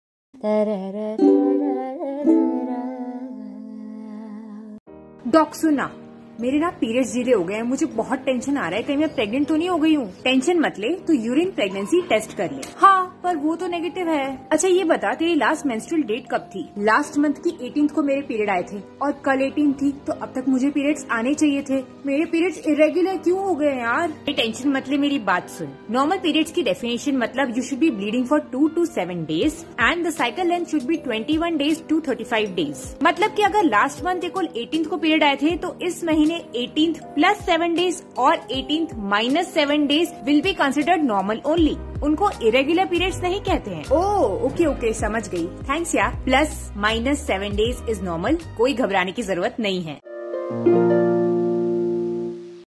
Is Irregular Periods Normal? | Dr. Isha Kapoor | Team GIRL-O-LOGY | Health Tips
Are you also confused or are you in doubt about you irregular periods? Let's talk about it. What our doctor has to say about irregular periods?

Speaker:
Dr. Isha Kapoor
(Obstetrician-Gynecologist)